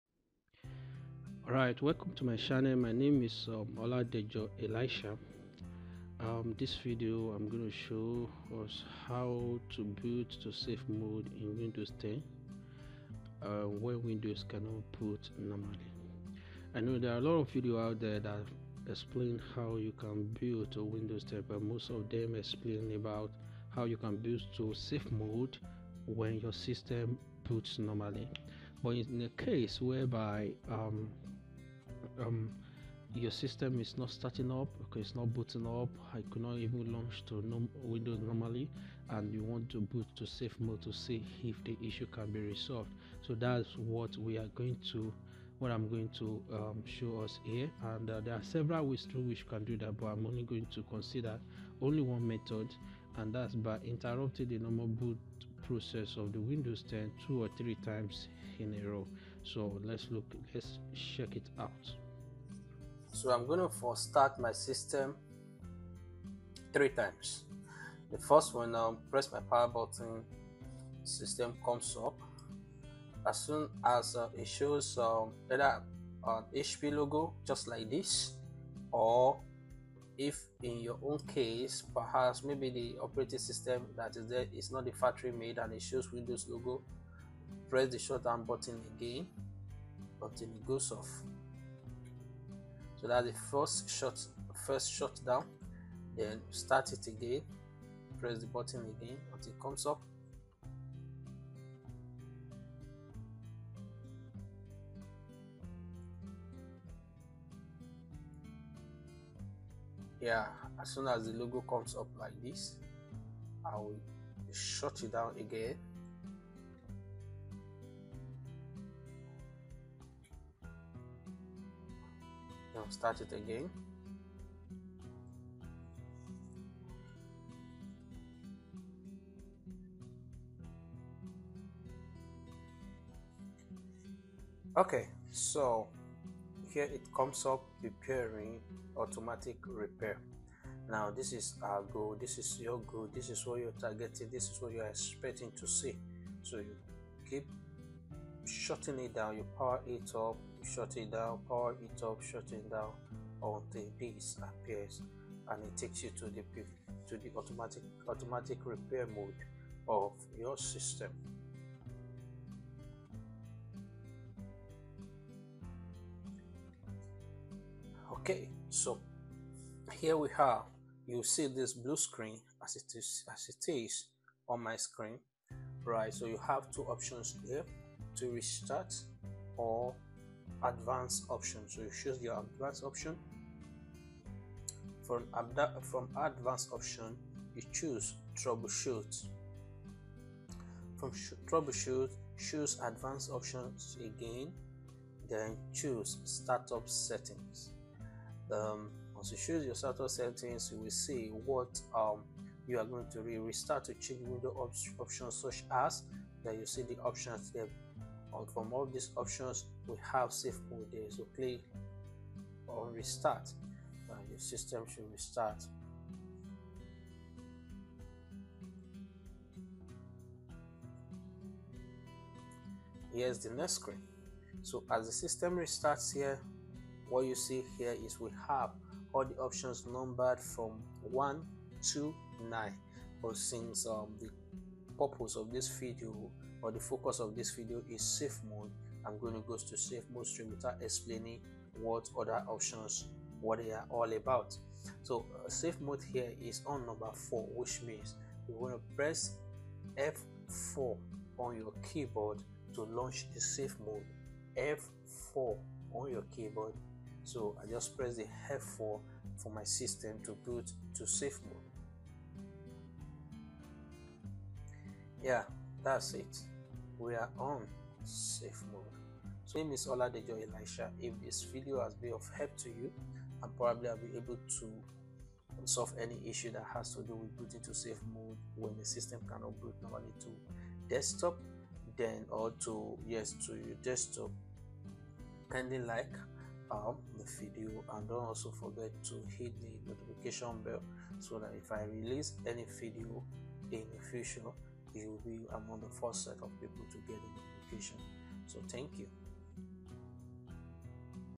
How to boot to safe mode windows 10 | When Windows Cannot Boot Normally
Knowing how to boot to safe mode windows 10 can be a great way to troubleshoot or diagnose problems on Windows 10 PC. Merely pressing F8 to get into Safe Mode doesn't work on Windows 10.
This video shows how to boot to safe for diagnoses when windows cannot boot normally.